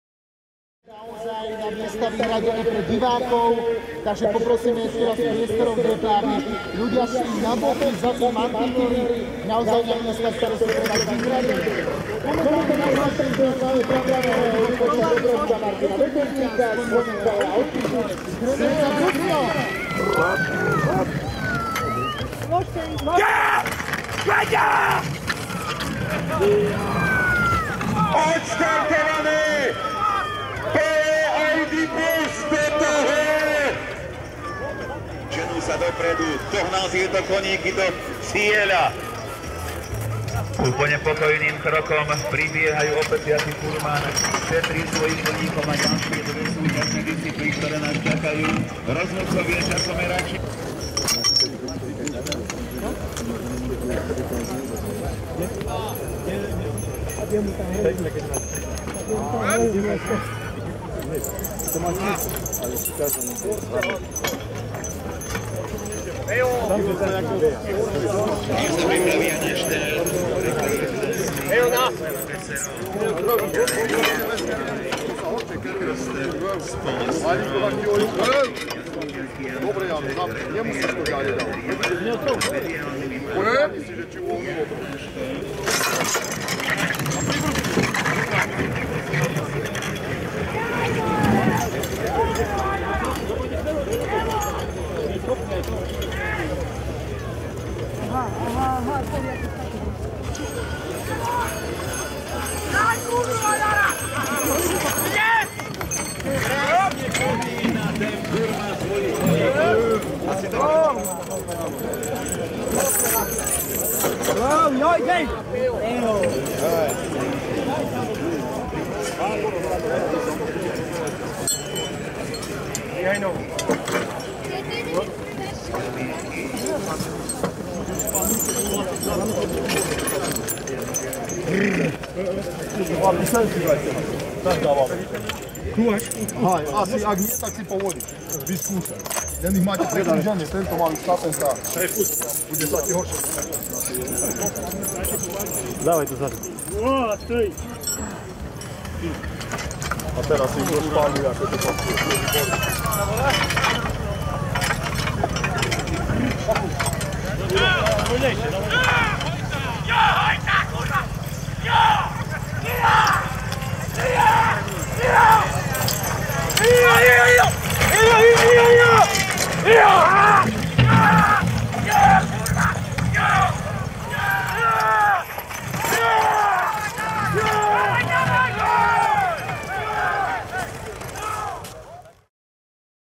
Furmanské preteky v Hriňovej
Malá koštovka zo včerajšieho príjemného dňa v Hriňovej.
Viac bude na filmovom festivale. Možno...  :-)